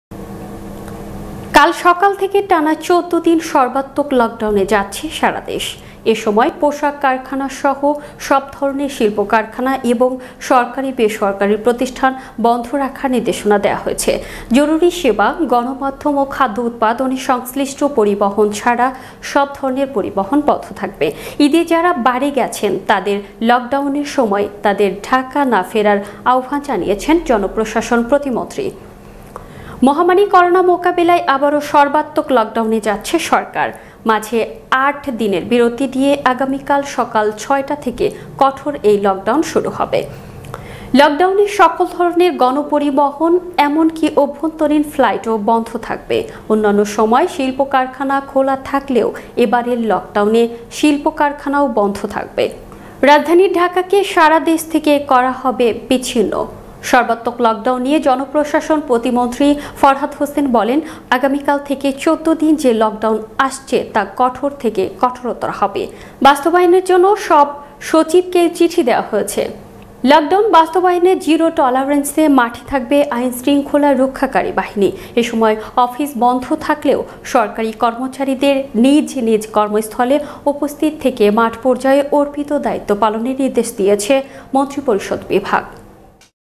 ঈদের পরদিন এবং লকডাউনের আগের দিন অনেকেই ঢাকা ছাড়ছেন আবার ফিরছেনও
কাল সকাল থেকে টানা ১৪ দিন সর্বাত্নক লকডাউনে যাচ্ছে সারা দেশ। এ সময় পোশাক কারখানাসহ সব ধরনের শিল্পকারখানা এবং সরকারি-বেসরকারি প্রতিষ্ঠান বন্ধ রাখার নির্দেশনা দেয়া হয়েছে। জরুরি সেবা, গণমাধ্যম ও খাদ্য উৎপাদনে সংশ্লিষ্ট পরিবহন ছাড়া সব ধরনের পরিবহন বন্ধ থাকবে। ঈদে যারা বাড়ি গেছেন তাদের লকডাউনের সময়ে তাদের ঢাকায় না ফেরার আহবান জানিয়েছেন জনপ্রশাসন প্রতিমন্ত্রী।
মহামারী করোনা মোকাবেলায় আবারো সর্বাত্নক লকডাউনে যাচ্ছে সরকার । মাঝে আট দিনের বিরতি দিয়ে আগামীকাল সকাল ৬ টা থেকে কঠোর এই লকডাউন শুরু হবে ।
লকডাউনে সকল ধরনের গনপরিবহন এমনকি অভ্যন্তরীন ফ্লাইটও বন্ধ থাকবে । অনান্য সময় শিল্পকারখানা খোলা থাকলেও এবারের লকডাউনে শিল্পকারখানও বন্ধ থাকবে । রাজধানী ঢাকাকে সারা দেশ থেকে করা হবে বিছিন্ন ।
সর্বাত্মক লকডাউন নিয়ে জনপ্রশাসন প্রতিমন্ত্রী ফরহাদ হোসেন বলেন আগামীকাল থেকে  ১৪ দিন যে লকডাউন আসছে তা কঠোর থেকে কঠোরতর হবে। বাস্তবায়নের জন্য সব সচিবকে চিঠিও দেওয়া হয়েছে ।
লকডাউন বাস্তবায়নে জিরো টলারেন্সে মাঠে থাকবে  আইন-শৃঙ্খলা রক্ষাকারী বাহিনী। এসময়  অফিস বন্ধ থাকলেও সরকারি কর্মচারীদের নিজ নিজ কর্মস্থলে উপস্থিত থেকে মাঠপর্যায়ে অর্পিত দায়িত্ব পালনের নির্দেশ দিয়েছে মন্ত্রিপরিষদ বিভাগ।